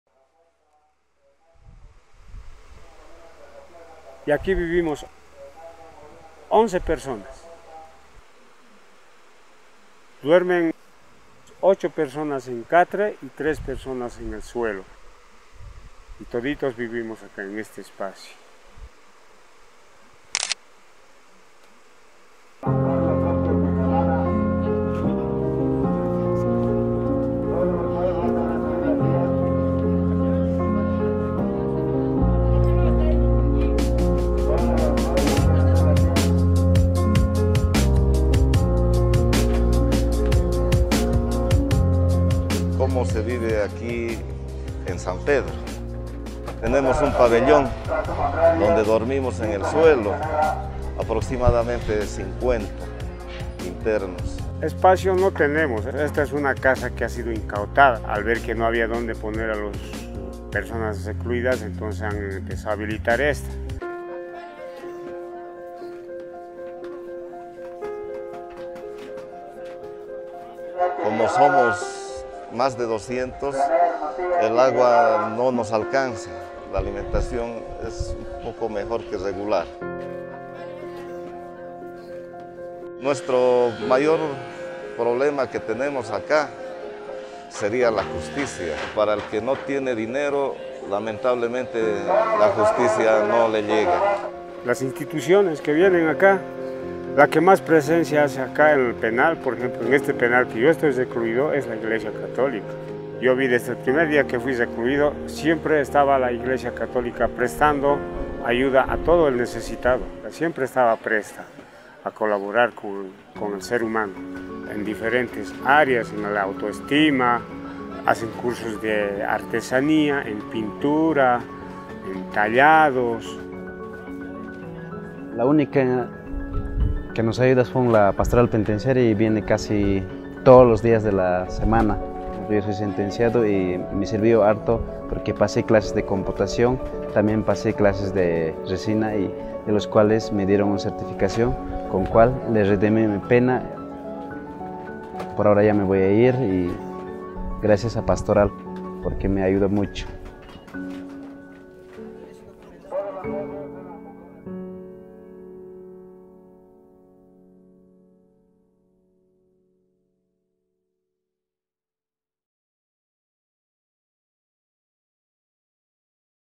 AdV 4 - Centro Penitenciario San Pedro de Sacaba
En la cárcel de Sacaba, los internos se enfrentan a diario a un entorno insalubre, una alimentación deficiente y unas condiciones de hacinamiento insostenibles. Misevi trabaja en el penal para mejorar esta preocupante situación de pobreza.